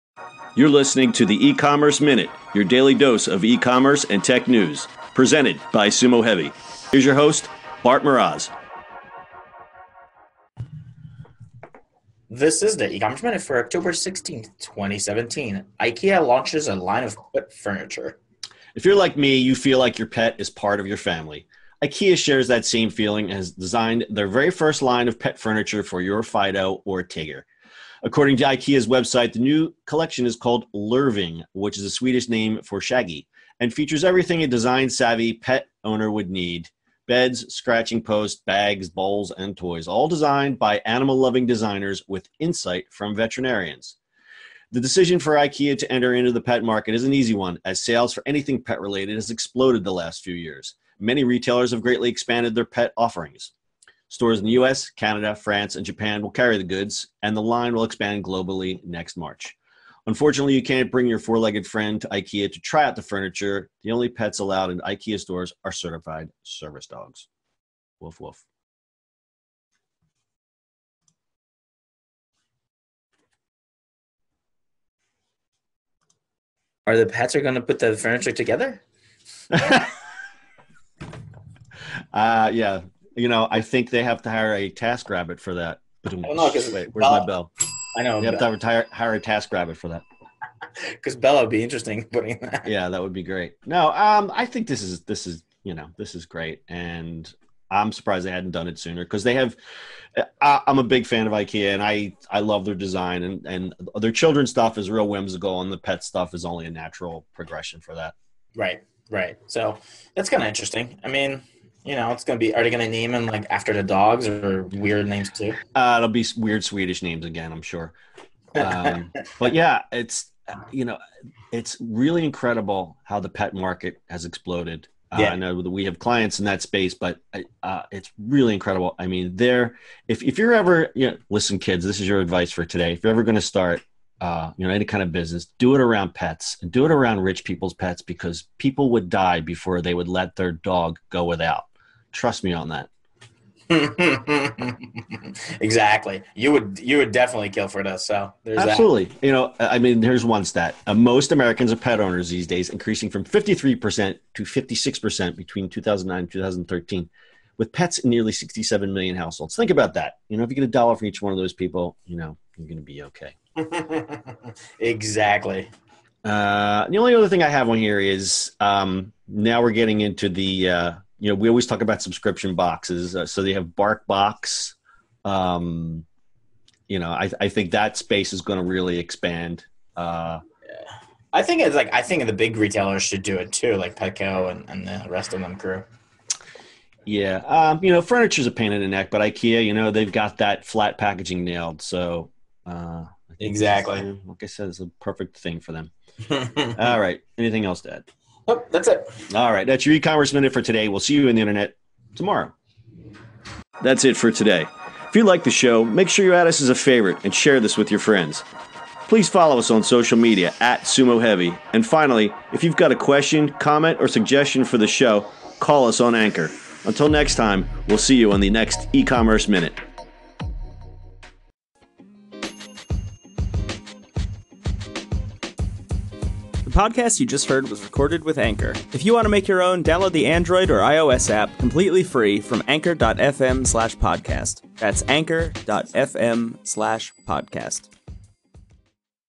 10.16.17: Ikea's 'Lurvig' Line of Pet Furniture
Ikea launches 'Lurvig' line of pet furniture